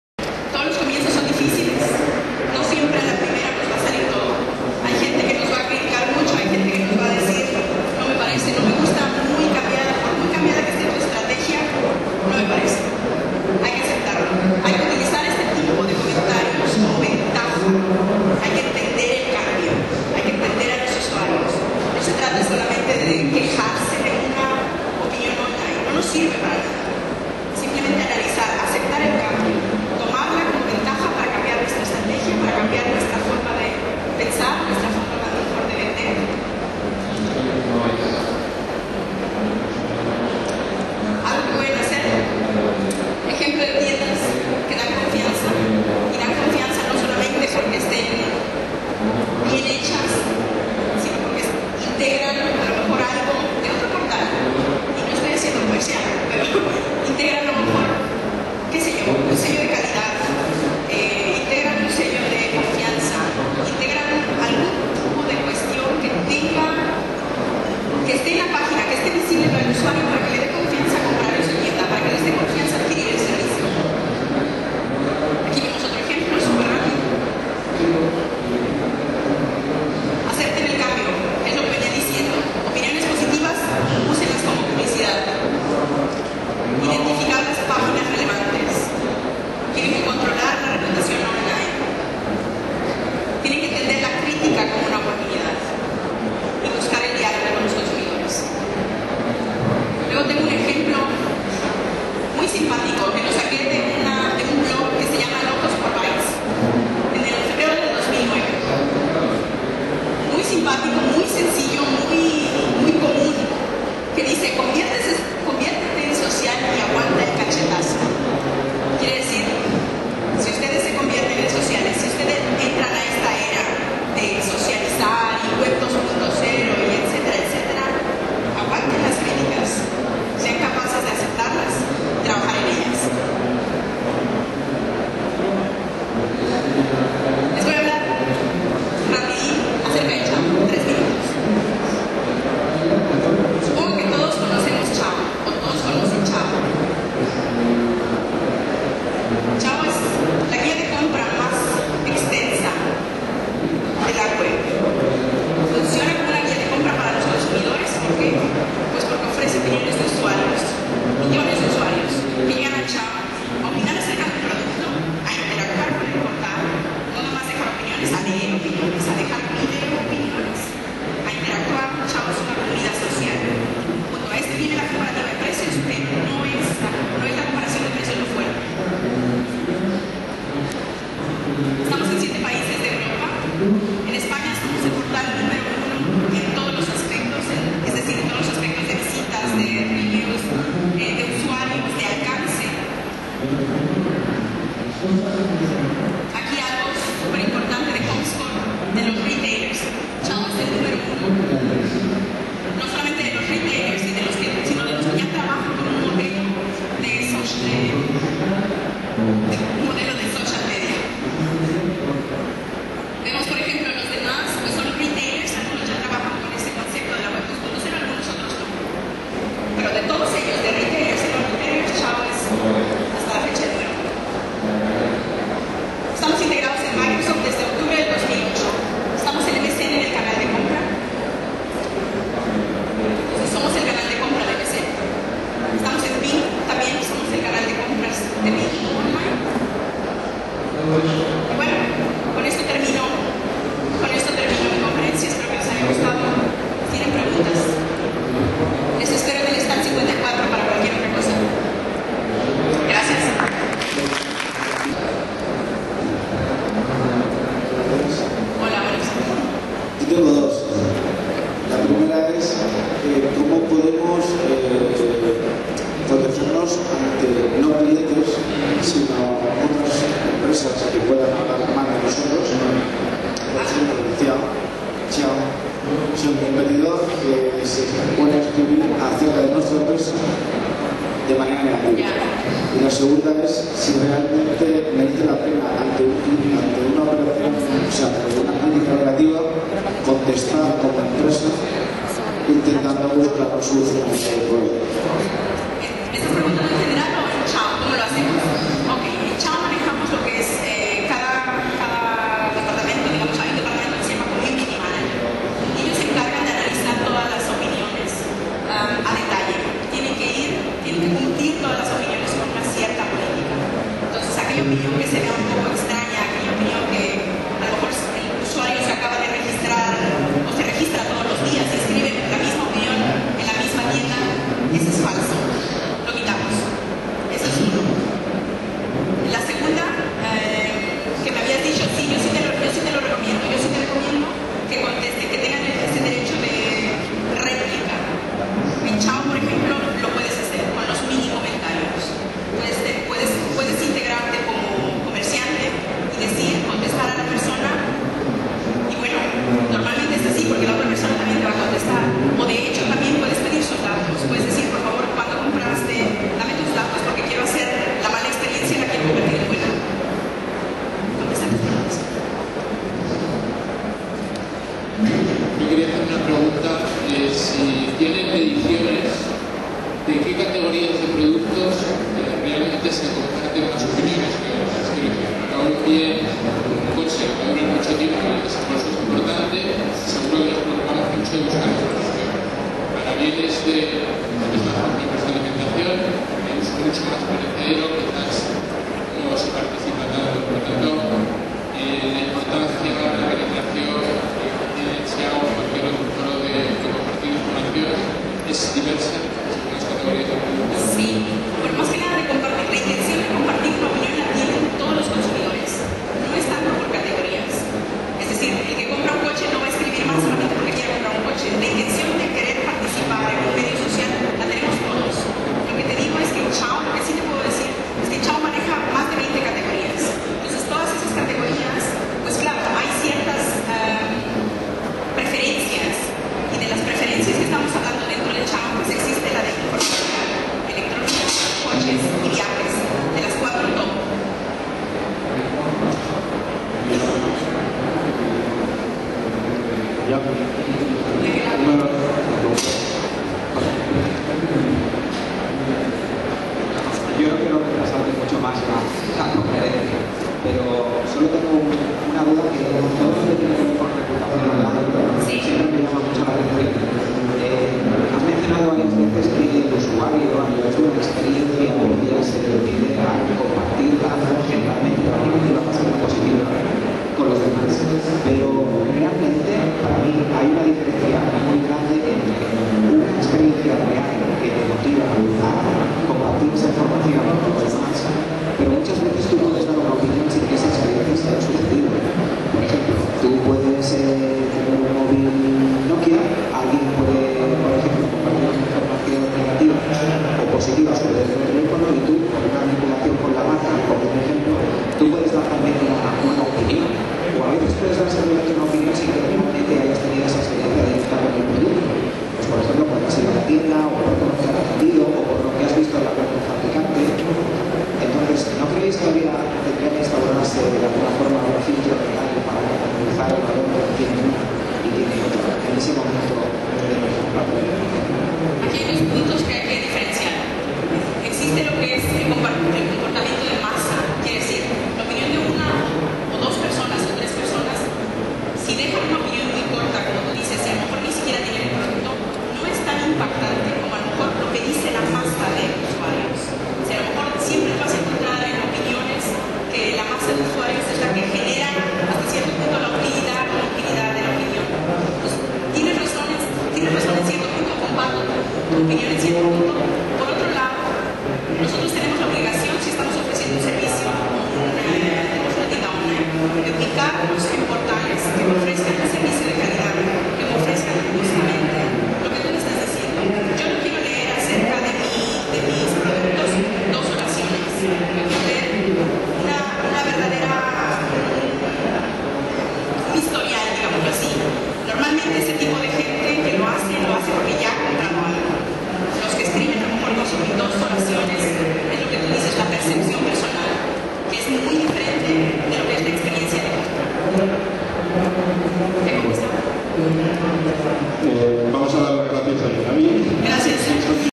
Reputación Online: Las opiniones de los usuarios como la clave del éxito
Durante la mañana del segundo día, Yaravi López-Petersen, Team Leader Sales Ciao e-Commerce Spain, dio una aclamada charla sobre la Reputación Online: Las opiniones de los usuarios como la clave del éxito. La sala de la conferencia estaba abarrotada, y mucha gente quiso preguntarle muchos detalles y ampliar sus conocimientos con sus ideas y recomendaciones sobre el tema en sí. Como se destacó en la charla, las opiniones de los usuarios y sus recomendaciones influyen más en las decisiones de compra que los anuncios online o los propios dependientes, de ahí la importancia de la reputación online.